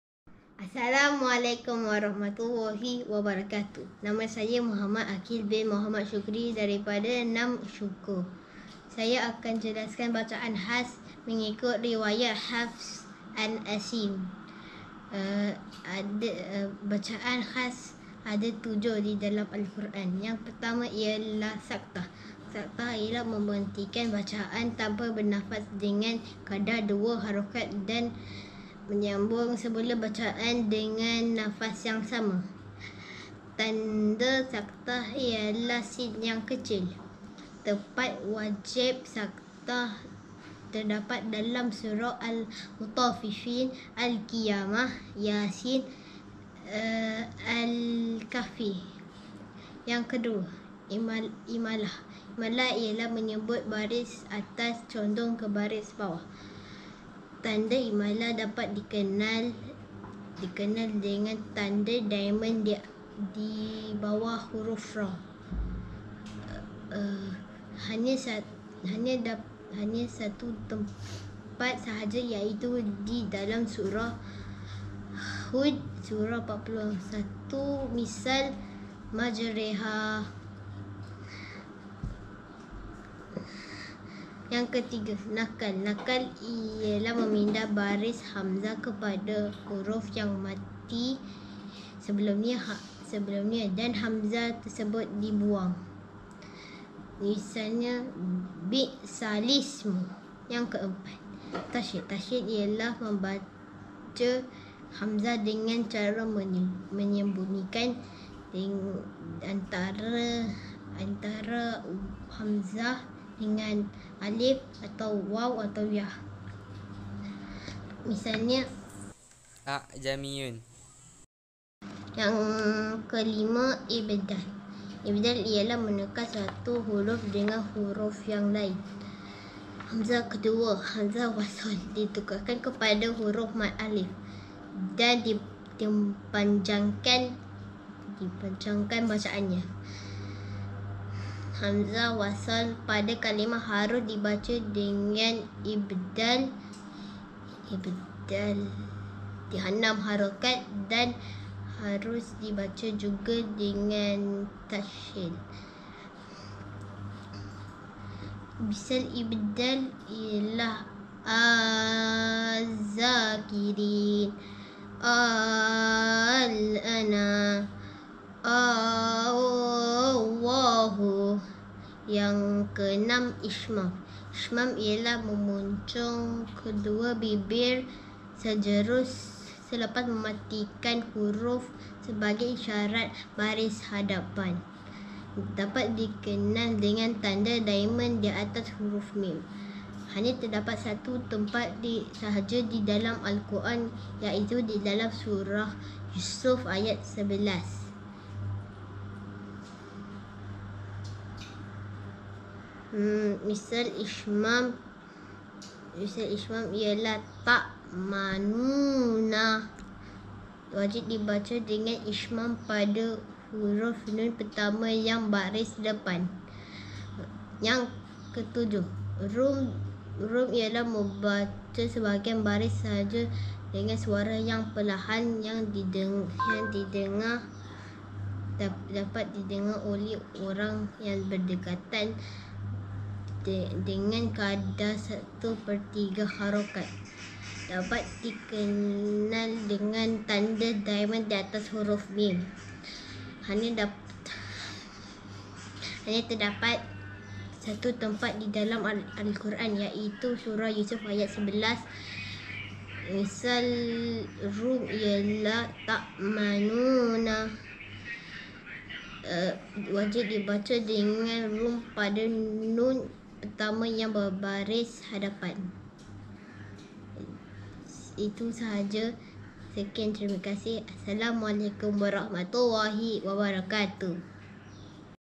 Bacaan wajib dalam Al Quran mengikut riwayat Hafs An Asim.
Latihan sekolah rendah agama sepanjang PKP. ini antara satunya latihan hafalan. Suruh hafal x hafal2 jadi kite kate la kena buat vedio cikgu nak cek... Baru la bersungguh2 nak hafal. Ini bukan untuk mengajar tapi sekadar simpanan vedio di sini.